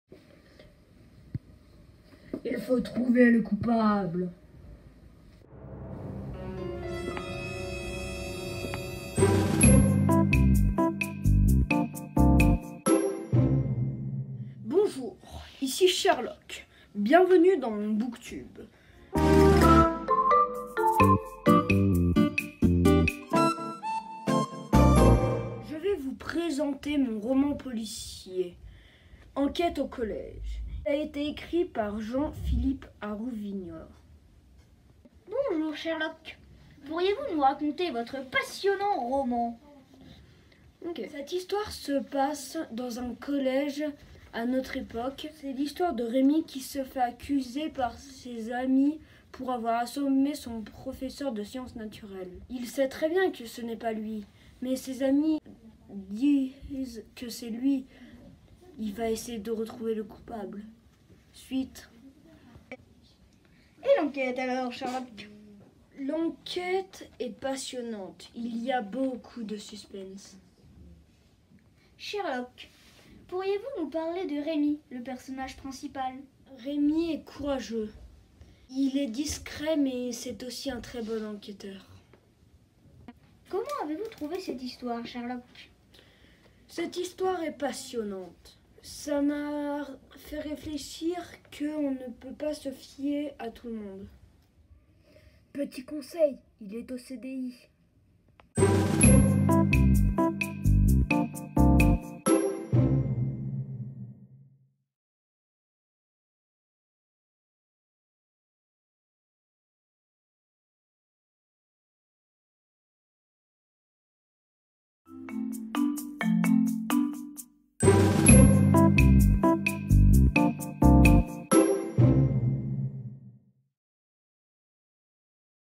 BOOKTUBE : Présentation du roman Enquête au collège de J.P Arrou-Vignod par Sherlock.
Les Attrapeurs de livres, la chaîne de lecture du CDI (SAISON 2).
Activité menée lors du CLUB CDI.